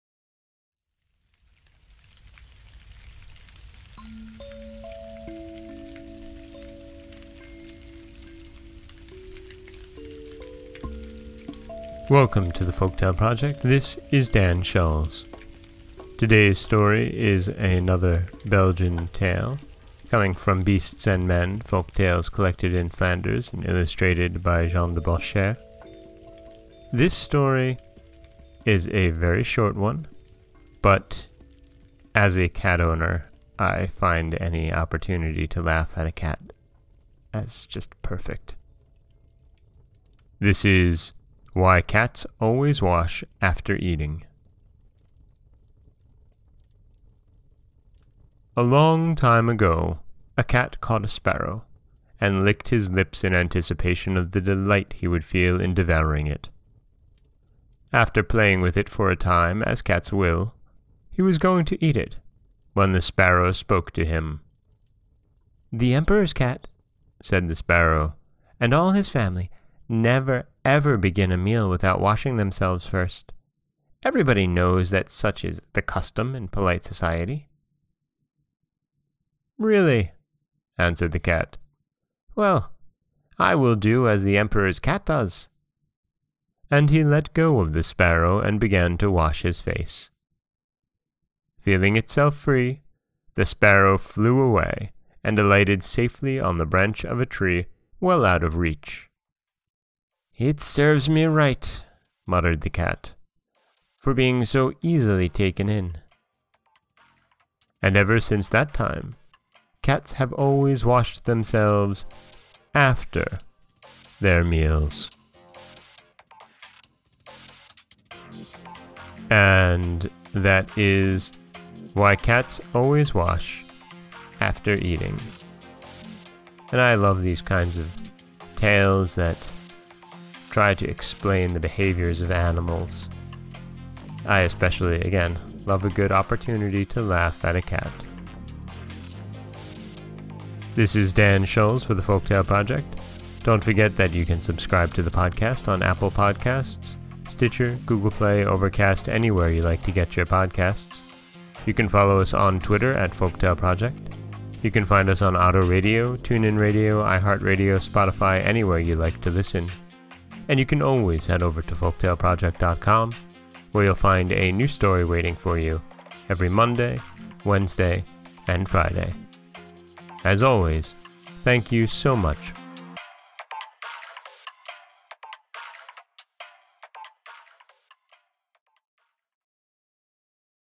Why Cats Always Wash After Eating
A long time ago a cat caught a sparrow, and licked his lips in anticipation of the delight he would feel in devouring it. After playing with it for a time, as cats will, he was going to eat it, when the sparrow spoke to him.   &nbsp; “The Emperor’s cat,” said the sparrow, “and all his family, never begin a meal without washing themselves first. Everybody knows that such is the custom in polite society.”